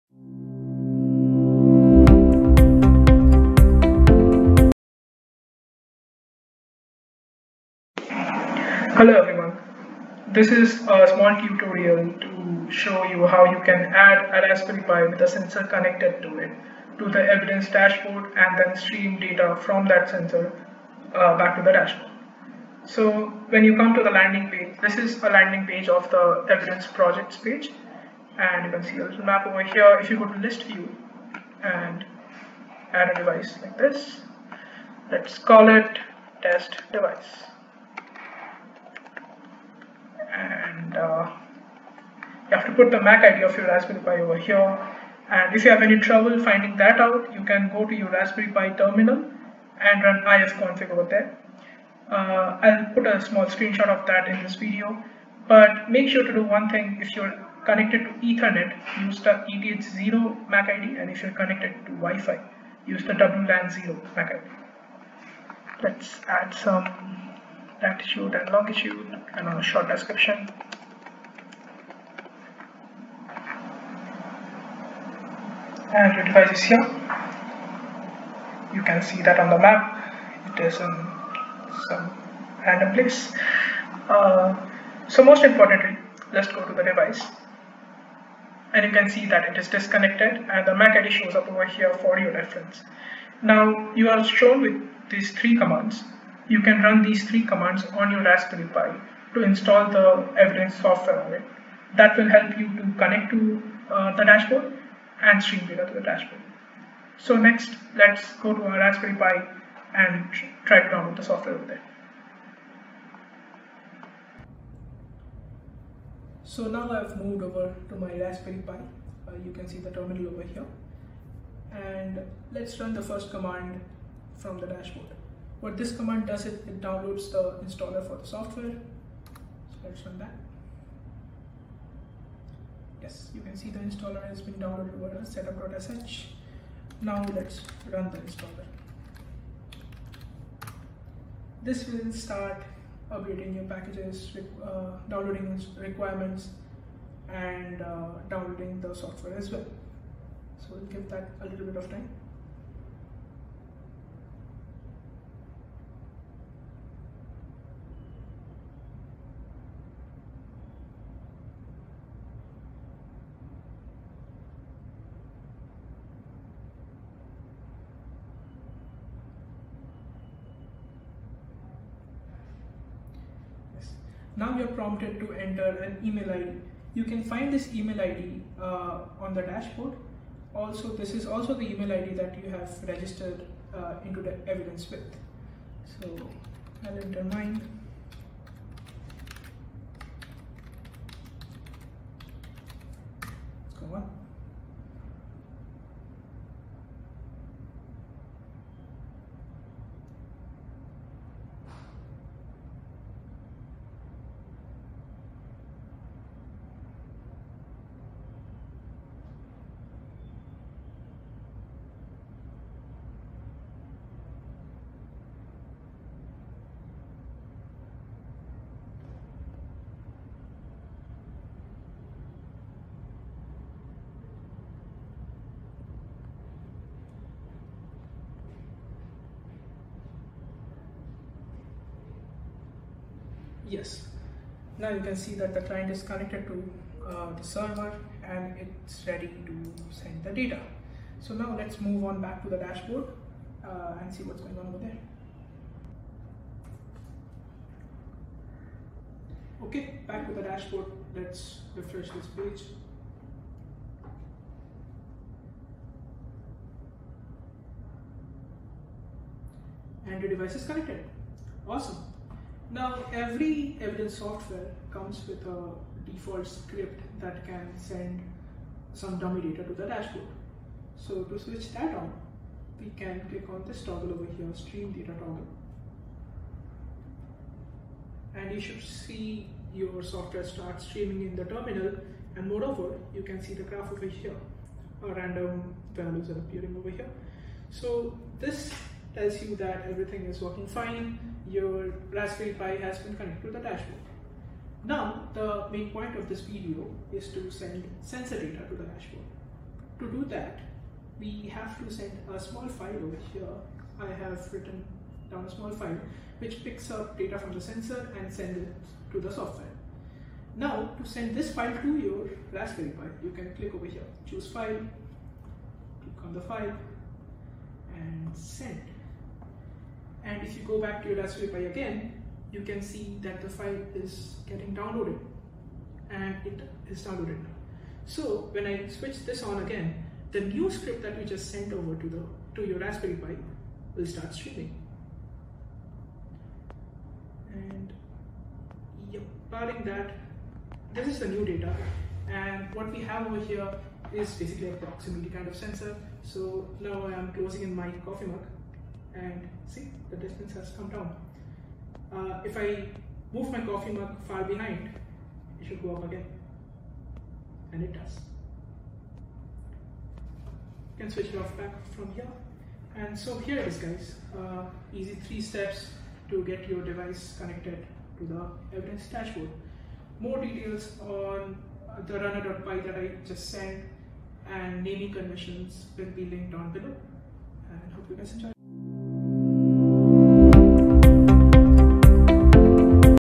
How to build an IOT Dashboard in 5 mins with Raspberry Pi and Evidence
Imagine a scenario, you have a number of IoT devices that are able to gather sensor data. Wouldn't it be nice if there was a way to visualize all this data in real-time, get back historical data when you need it and have the ability to run ML algorithms on that data? This is what EVIDence is all about.

EVIDence is an IoT dashboard that is capable of streaming and graphically representing IoT sensor data. It is also capable of running ML algorithms on grid edge sensor data to predict if an EV is connected to the grid.

to know more about us!